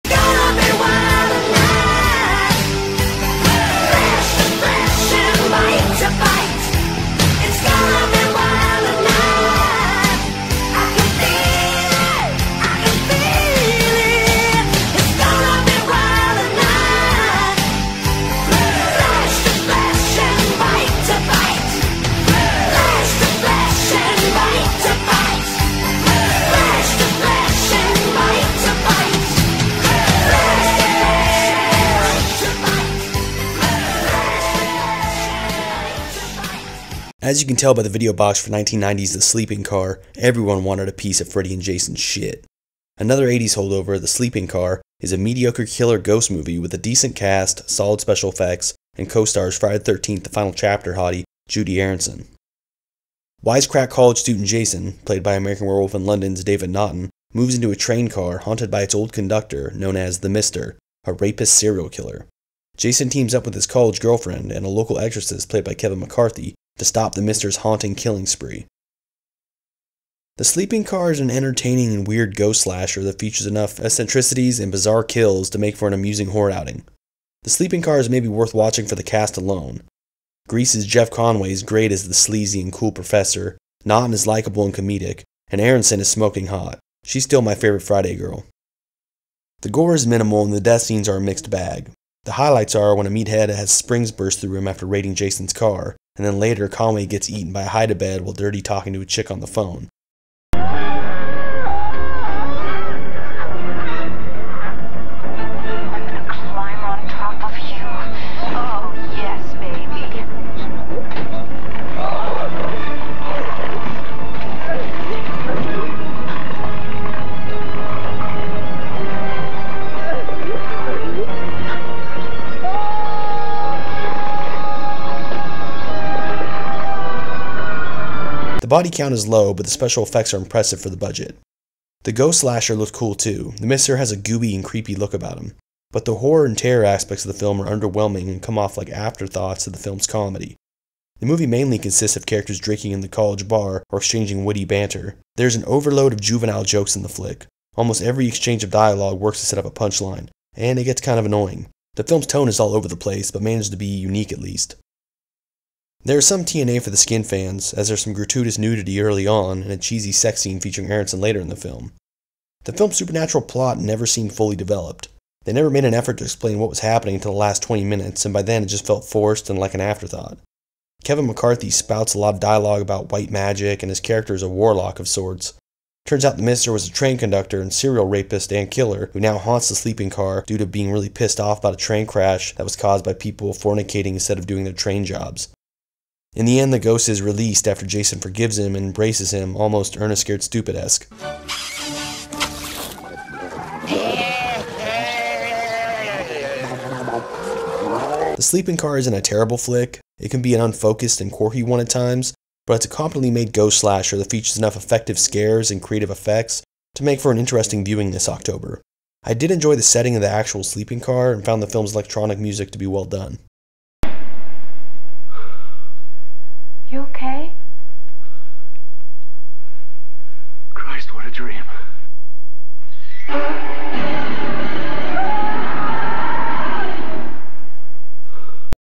The Sleeping Car (1990) 90's Horror Movie Review
Review of the ghost slasher The Sleeping Car from 1990 starring Jeff Conway, Judie Aronson, and David Naughton